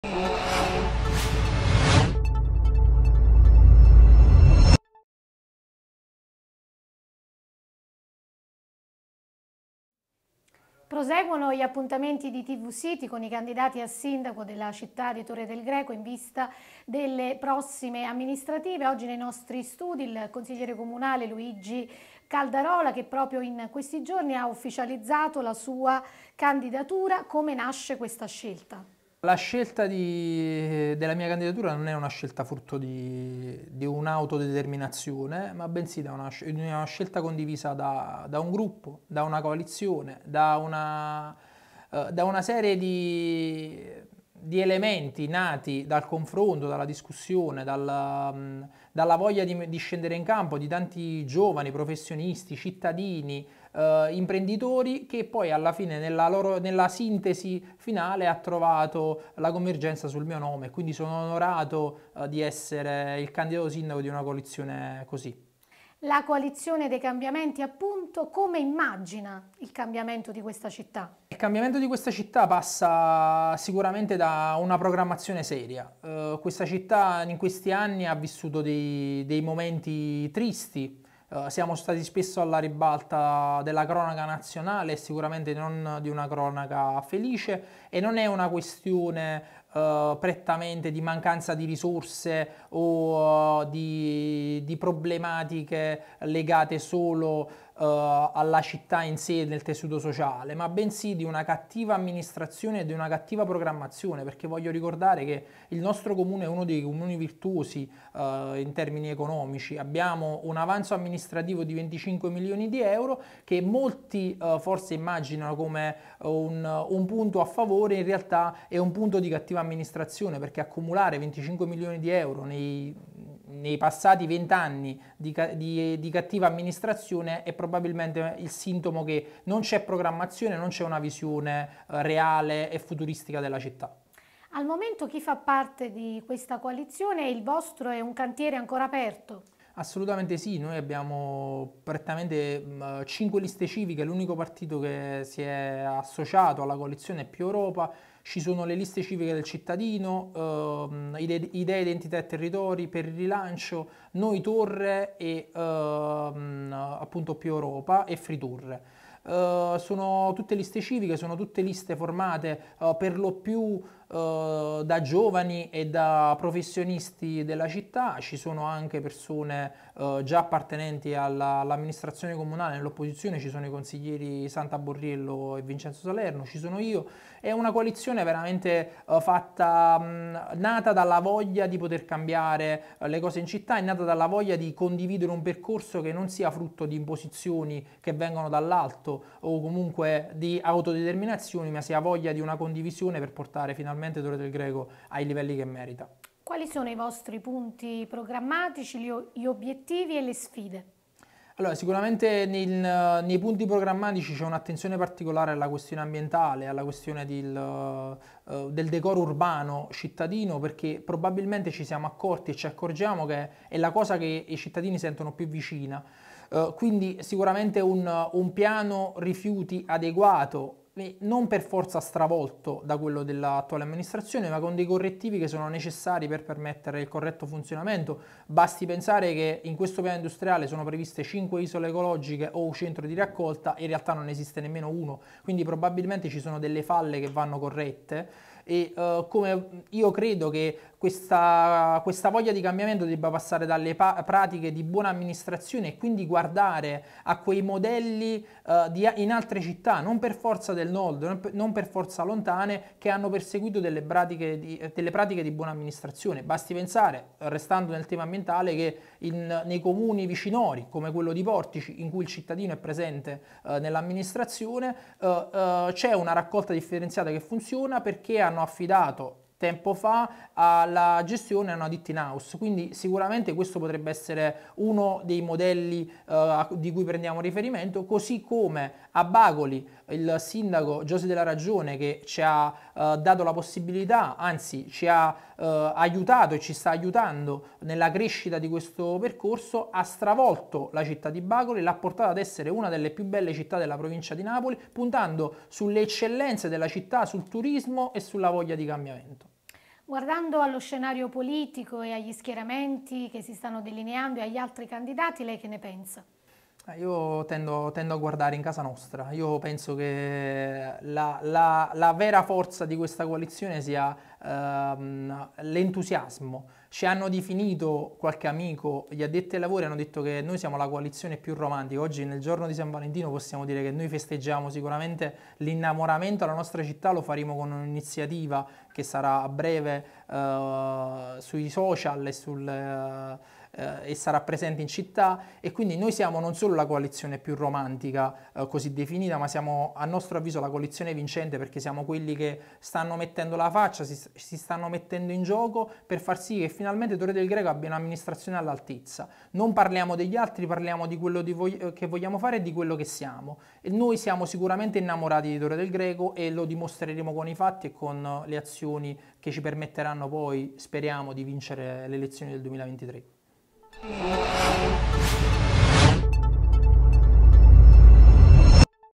Amministrative in città, l'intervista al candidato sindaco Luigi Caldarola
Proseguono gli appuntamenti di TVCITY con i candidati a sindaco della città di Torre del Greco, in vista delle prossime amministrative.

Questa mattina negli studi della nostra emittente il consigliere comunale Luigi Caldarola che di recente ha ufficializzato la sua candidatura.

"La mia è una scelta condivisa da un gruppo, da una coalizione.

Il cambiamento di questa città passa attraverso una programmazione seria".